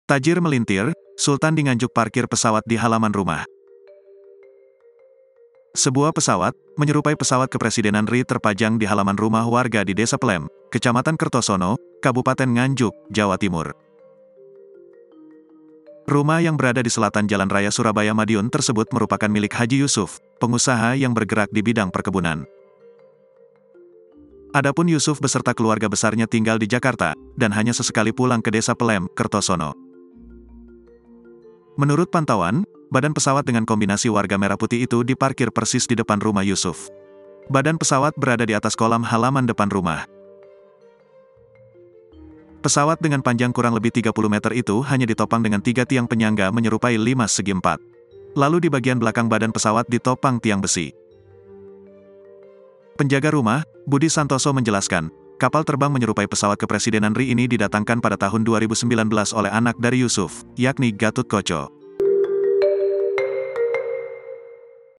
TAJIR MELINTIR, SULTAN DI NGANJUK PARKIR PESAWAT DI HALAMAN RUMAH
TRIBUNLAMPUNG.CO.ID - Sebuah pesawat, menyerupai pesawat kepresidenan RI terpajang di halaman rumah warga di Desa Pelem, Kecamatan Kertosono, Kabupaten Nganjuk, Jawa Timur.
Rumah yang berada di selatan Jalan Raya Surabaya-Madiun tersebut merupakan milik Haji Yusuf, pengusaha yang bergerak di bidang perkebunan.
Adapun Yusuf beserta keluarga besarnya tinggal di Jakarta, dan hanya sesekali pulang ke Desa Pelem, Kertosono.
Menurut pantauan, badan pesawat dengan kombinasi warga merah-putih itu diparkir persis di depan rumah Yusuf. Badan pesawat berada di atas kolam halaman depan rumah.
Pesawat dengan panjang kurang lebih 30 meter itu hanya ditopang dengan tiga tiang penyangga menyerupai limas segi empat. Lalu di bagian belakang badan pesawat ditopang tiang besi.